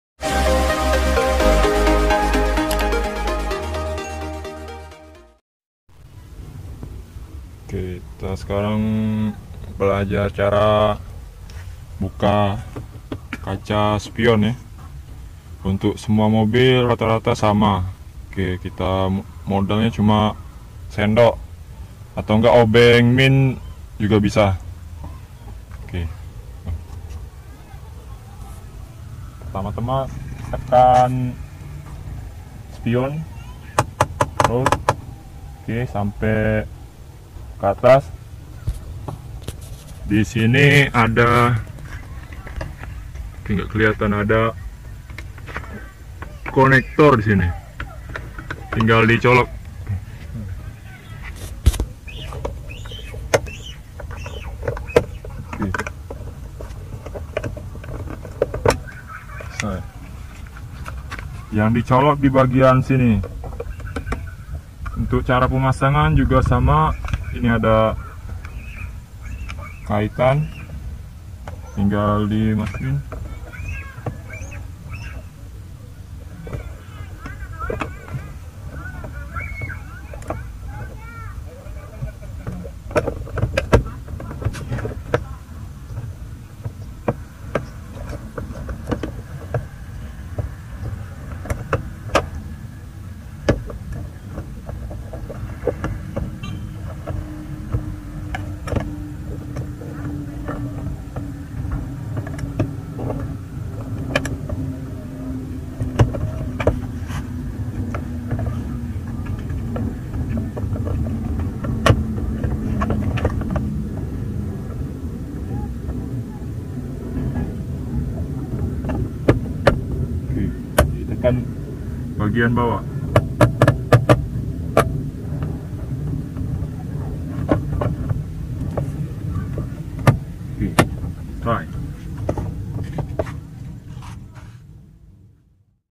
Ganti kaca spion avanza xenia sendiri
Mengganti kaca spion avanza atau xenia